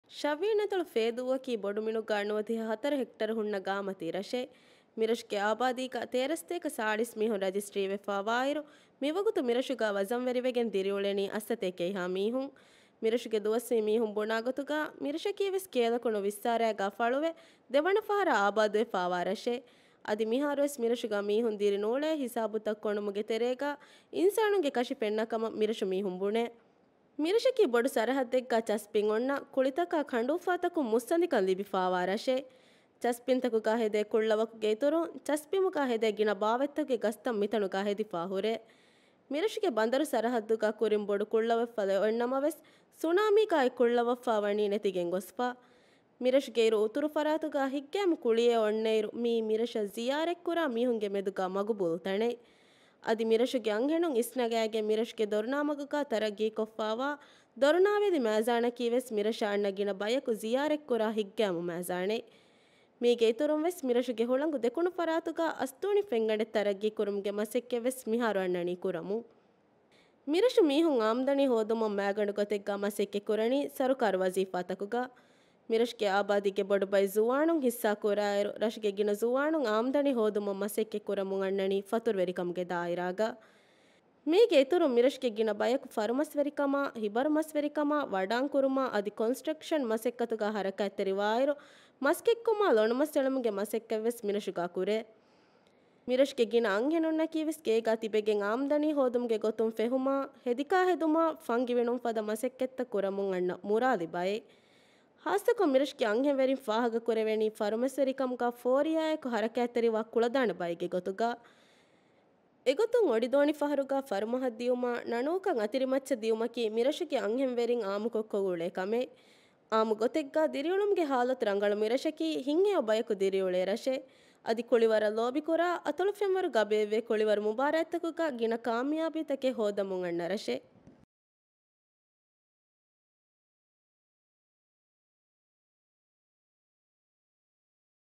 Island Profile - Sh. Feydhoo
Feydhoo (Dhivehi: ފޭދޫ) is one of the inhabited islands of the Shaviyani Atoll administrative division that is geographically part of the Miladhummadulhu Atoll in the Maldives. Feydhoo is located in the centre of the atoll between the channel called Noomara Kandu and the island Bileffahi.

00:00 - Intro and population
00:12 - Past settlement
00:27 - Wetland ecosystems
00:46 - Attractions
01:10 - Income and occupation
01:59 - Community